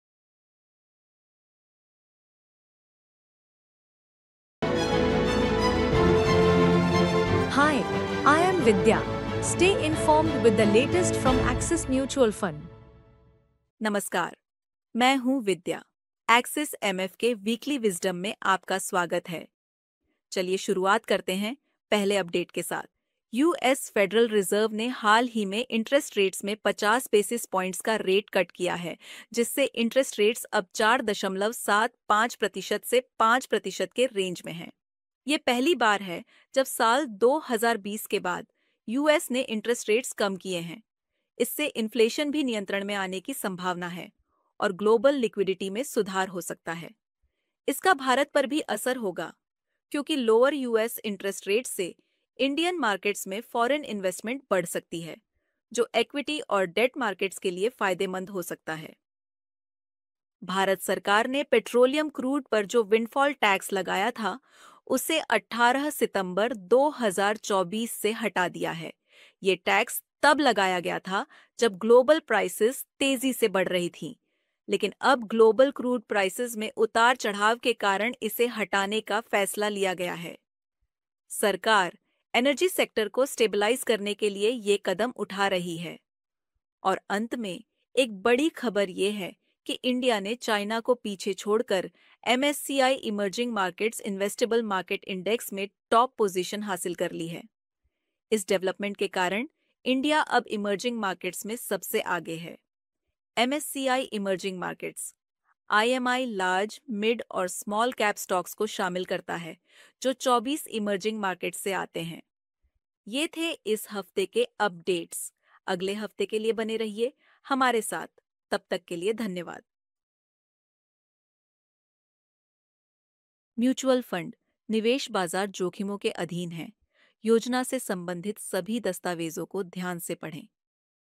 Weekly Wisdom with Vidya (September 19th, 2024 Edition) Axis Mutual Fund
In this video, you will get insights into the latest trends in the market and what they mean for mutual fund investors. Watch now to get up-to-date with the ever-changing investment landscape!

Follow us for more educational content and to stay updated with the world of investment. 📈

Explore various ways to connect with Axis Mutual Fund on our other pages: